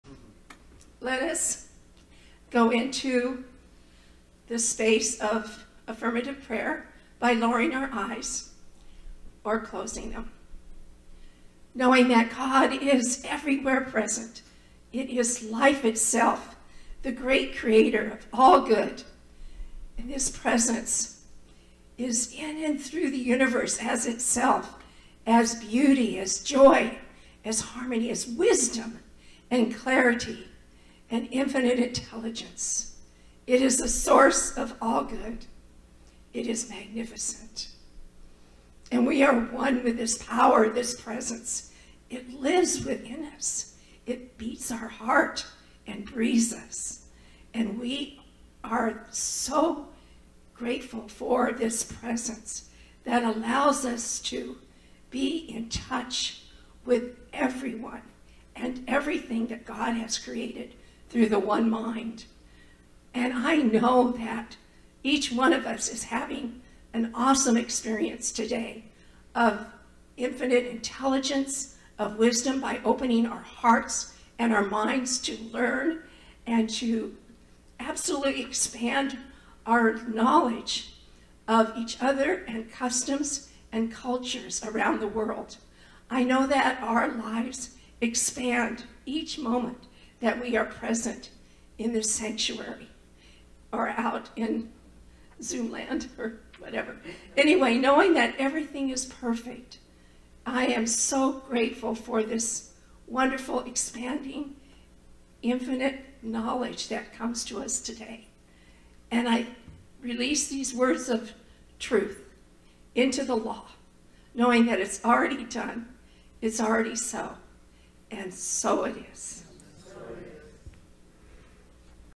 Toni Sparks, RScP - Prayer - 12/26/2021
Affirmative prayer with Toni Sparks, RScP with the Center for Spiritual Living Capistrano Valley in San Clemente, California.

Please consider making a donation to continue support of our Center using the following link: